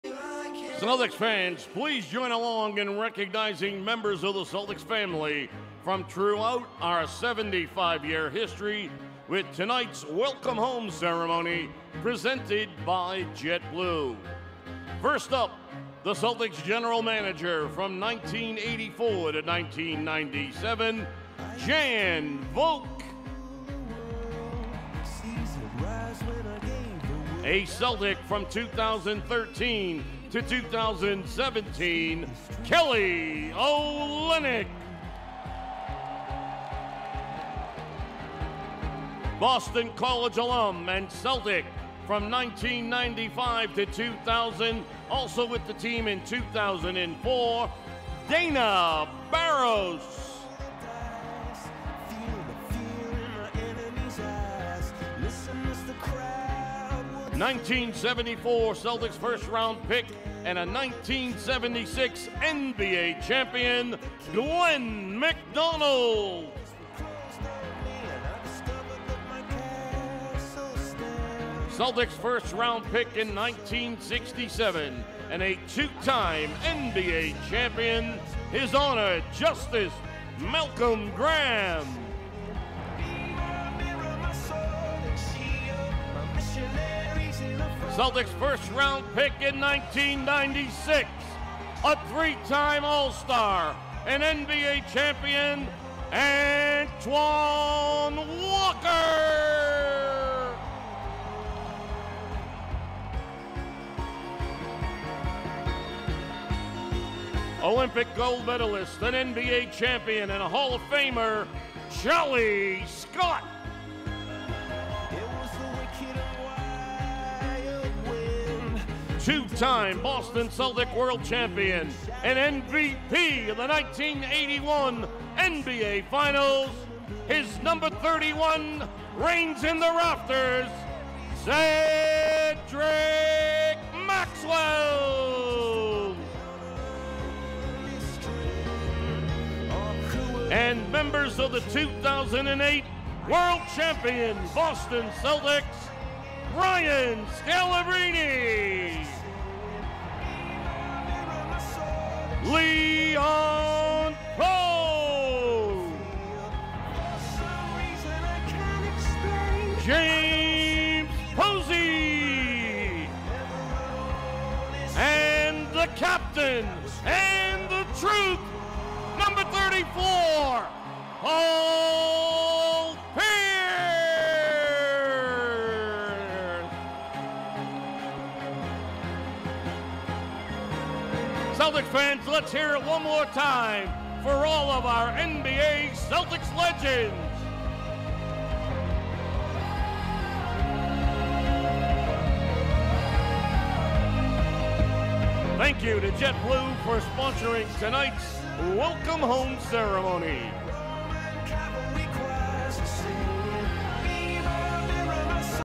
Boston Celtics Alumni Recognized In Welcome Home Night Halftime Ceremony at TD Garden
On the opening night of Welcome Home Weekend we celebrated the return of some of our favorite Celtics alumni at TD Garden.

Celtics from many different eras all held up their jerseys with pride. Returning members of the 2008 NBA Championship team included Brian Scalabrine, Leon Powe, James Posey and Paul Pierce, who all heard roars from the crowd upon introduction.